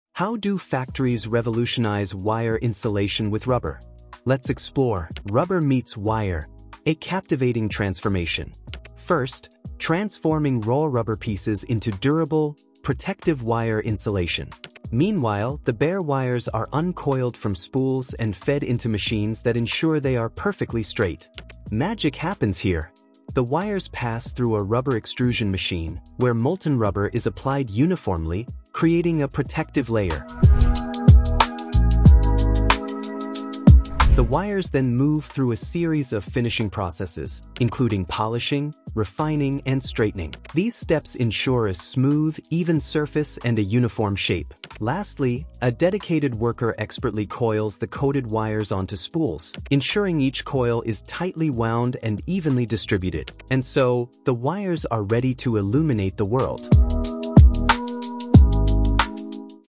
How Wire Insulation is Made from Rubber: Step-by-Step Process!
The video reveals the entire process of making wire insulation from rubber, offering superior durability, flexibility, and heat resistance. Rubber wire insulation ensures long-lasting protection against electrical hazards, making it ideal for both industrial and household applications. Moreover, this eco-friendly and reliable material enhances safety, reduces wear, and increases wire longevity in harsh environments.